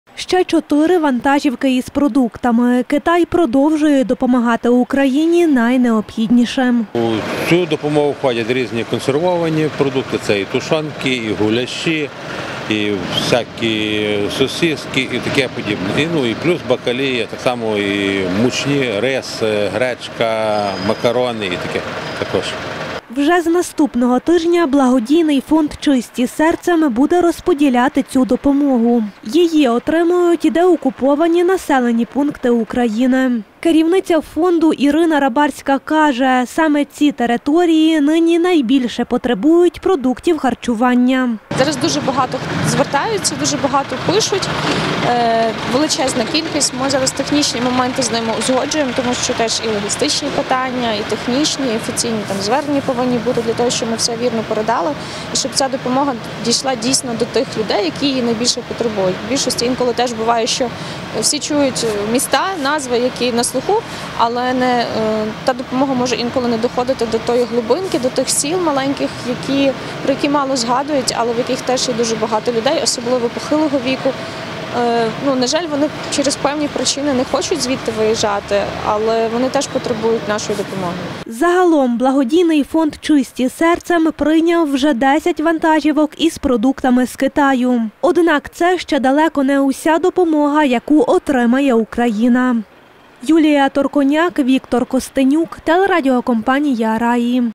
В Івано-Франківськ приїхали ще чотири фури з продуктами з Китаю
Доброчинці благодійного фонду "Чисті серцем" наразі розвантажують машини, а згодом - відправлятимуть цю допомогу в ті регіони України, які найбільше її потребують.